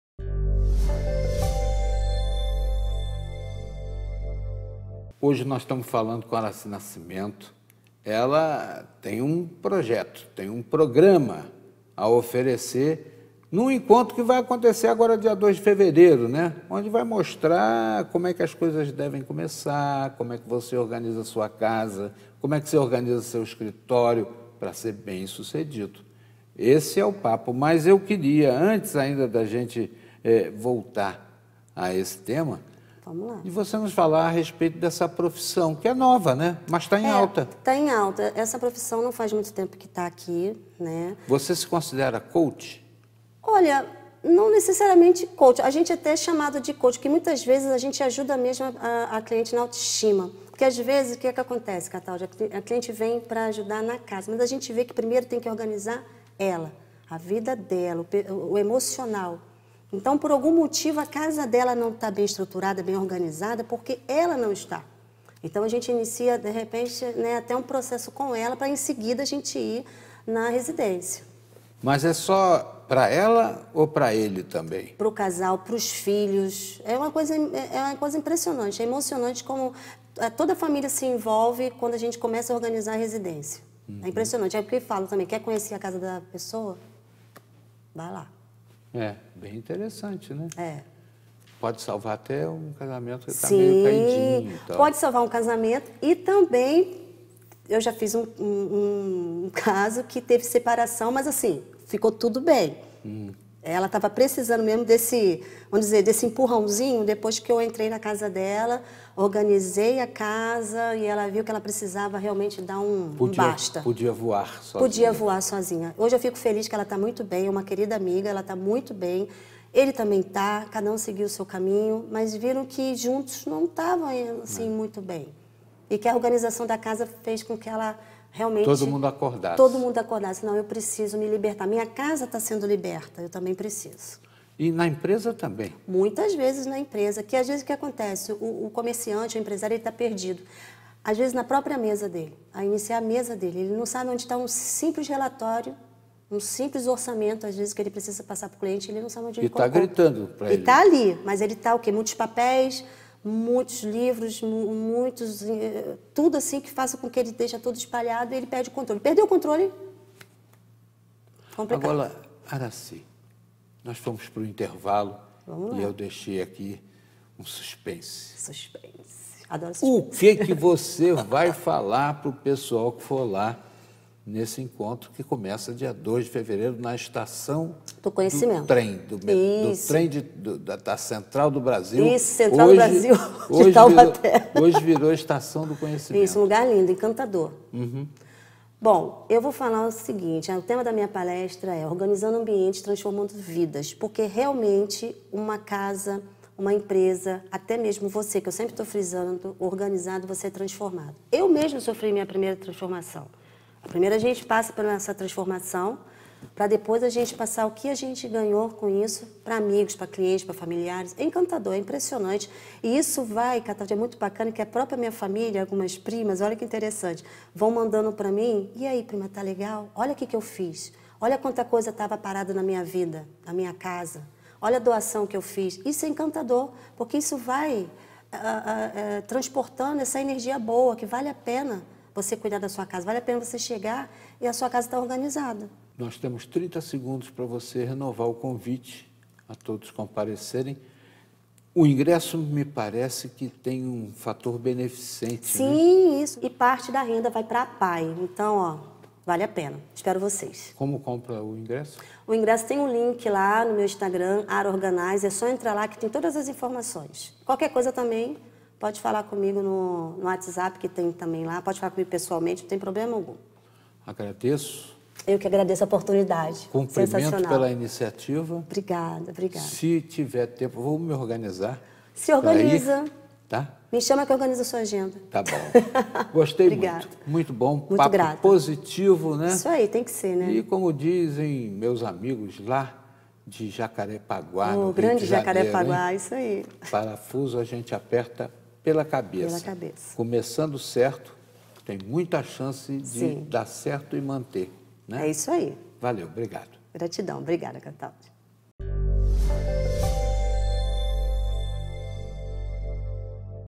Entrevista: como organizar a casa e o escritório (parte 2/2)
A personal organizer Araci Nascimento fala da profissão relativamente nova e dá dicas de como organizar a casa, o escritório e a vida para ter mais sucesso e alcançar os objetivos.